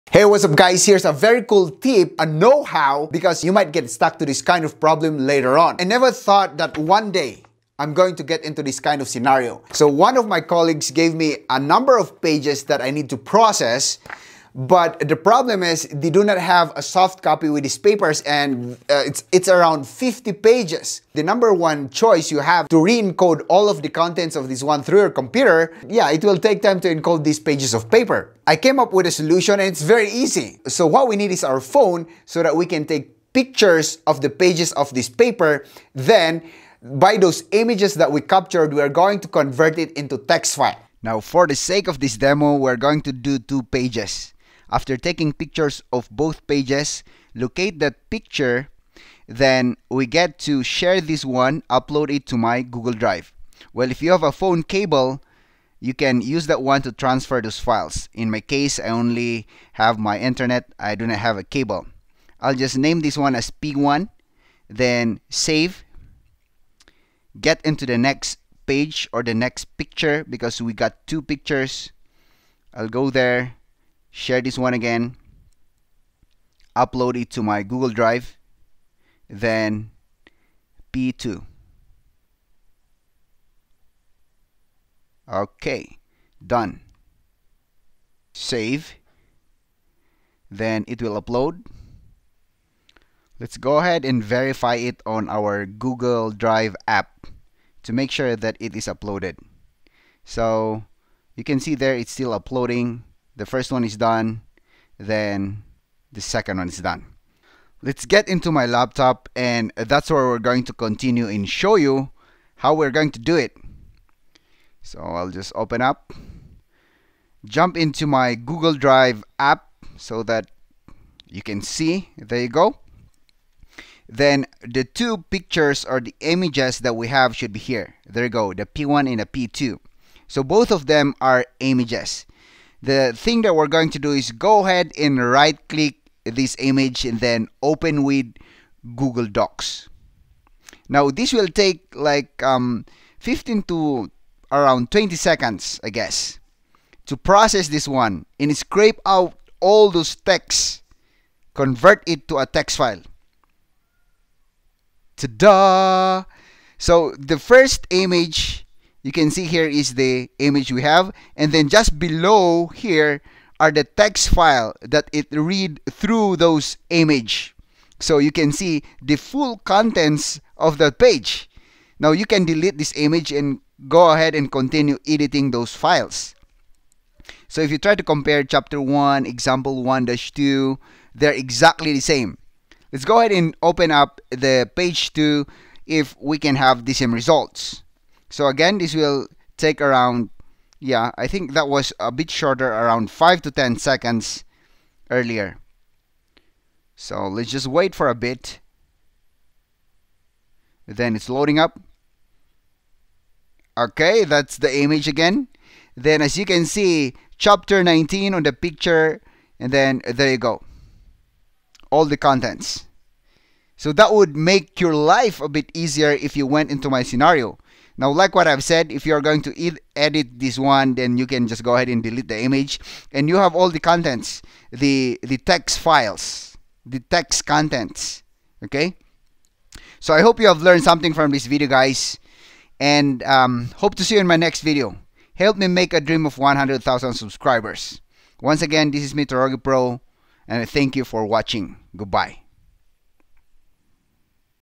How to Convert Image files (PNG, JPEG) to Editable text files! Free and Easy!
Today, I've got a super cool tip for you that can save you tons of time if you ever need to convert paper documents to text files. Imagine you have a stack of 50 pages with no soft copy—typing them all out would take forever! Don't worry, I've got a simple solution using just your phone and Google Drive. Let's dive in!

📱 In This Video:
*Problem Overview: Converting physical documents to text without manual typing.
*Quick Solution: Using your phone to take pictures of the documents.
*Step-by-Step Demo: Uploading images to Google Drive and converting them to text.
*Google Docs OCR: How to use Google Docs to extract text from images.
*Practical Example: Demonstrating the process with two sample pages.